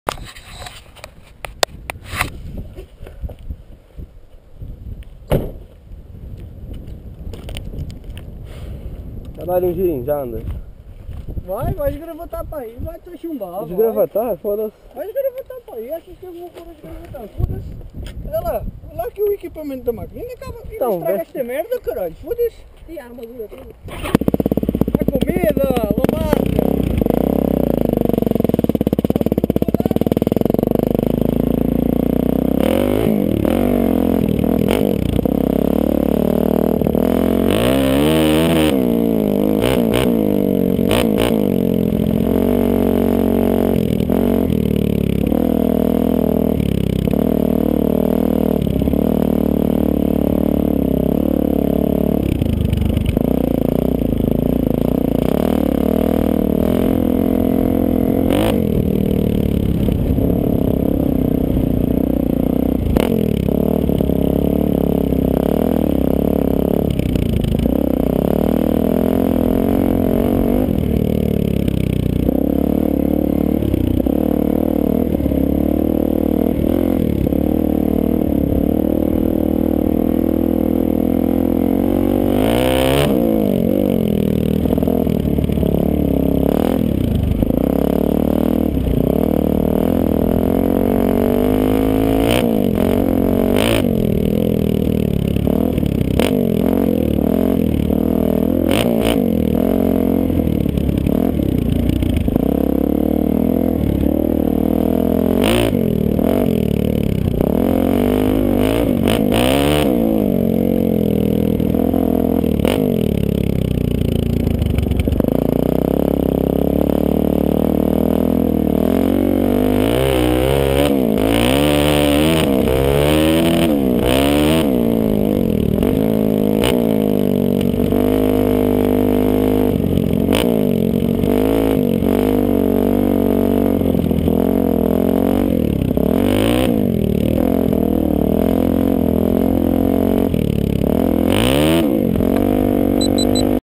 Dirt Bike [Suzuki Drz 400e] -Go Pro HD @ Lear
Go easy on the "nuub " comments =p  , its one of my first times riding a 2 wheel bike , i got it recently ! . Its a Suzuki Drz 400e from 2007 with Dynojet , Fmf power bomb , Fmf pipe , Air filter and some other stuff on it .